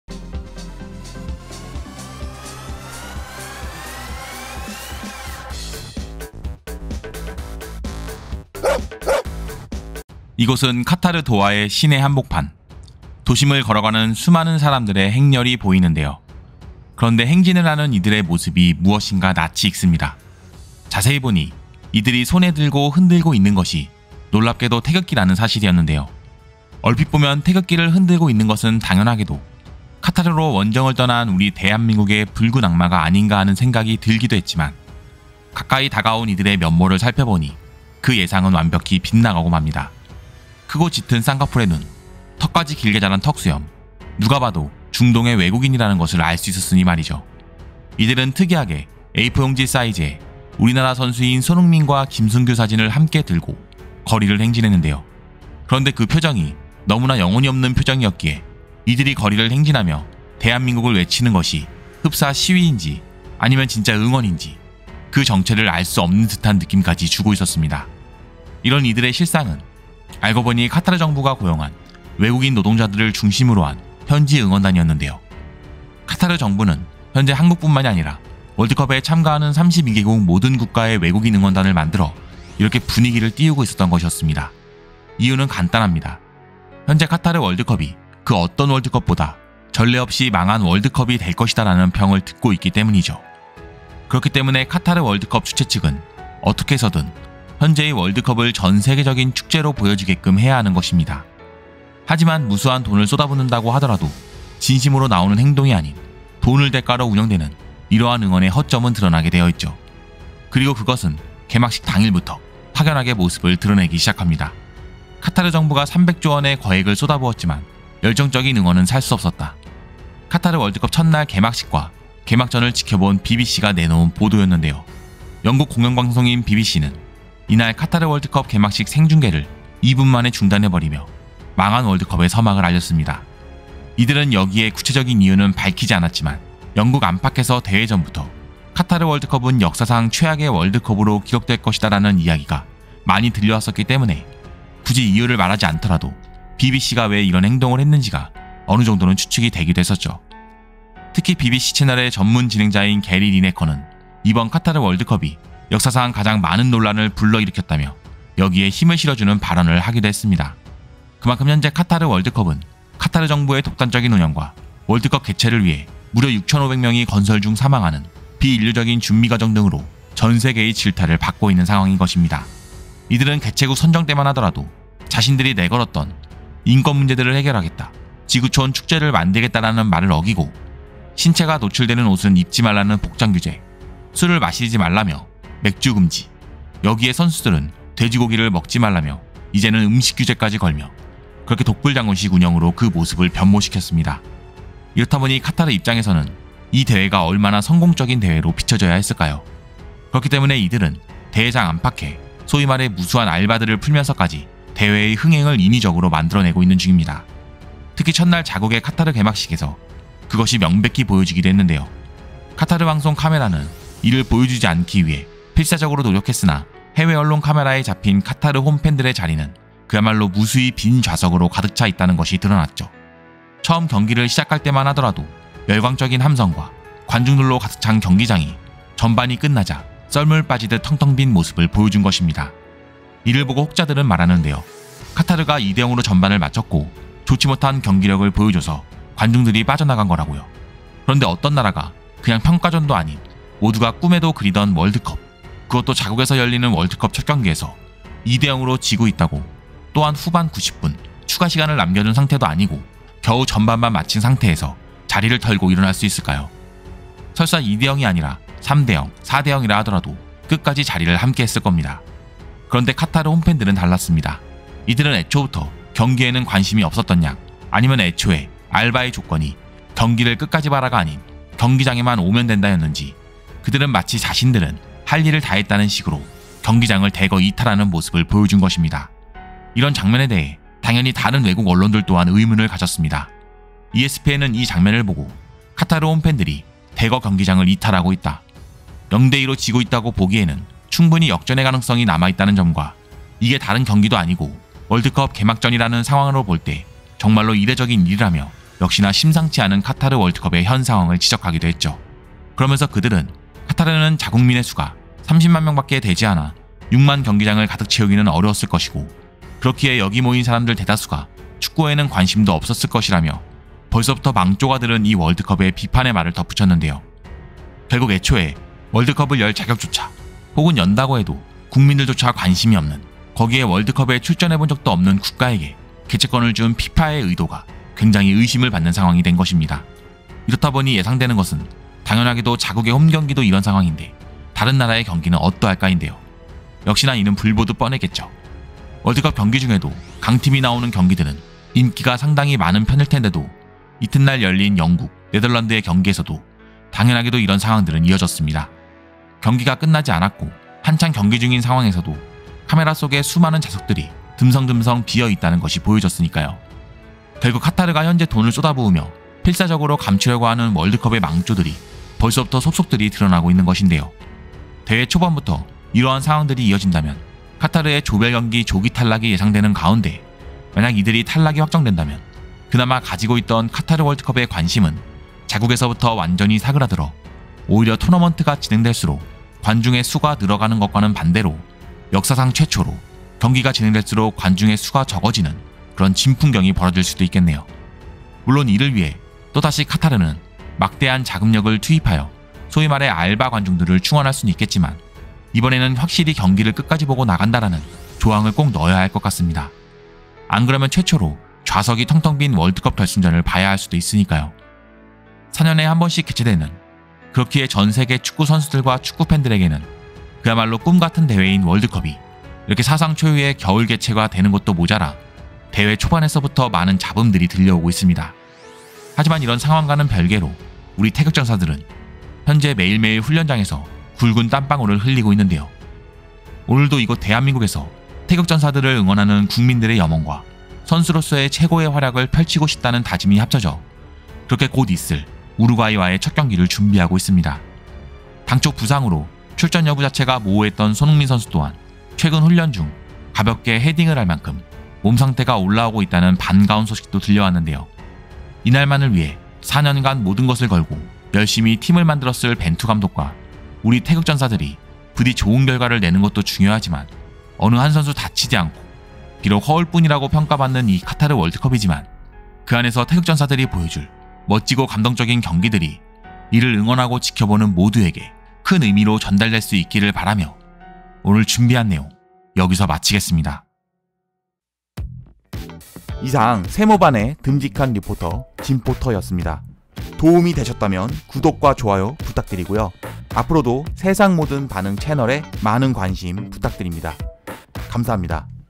(긴습 속보) 월드컵 생방송 중 카메라에 잡힌 기괴한 장면에 방송하던 BBC 앵커 2분만에 방송 꺼버린 이유
진포터 멤버십 중 일부 금액은 참전용사분들께 후원됩니다.

안녕하세요.
대한민국에 관한 세상 모든 반응을
모아 전달하는 진포터입니다.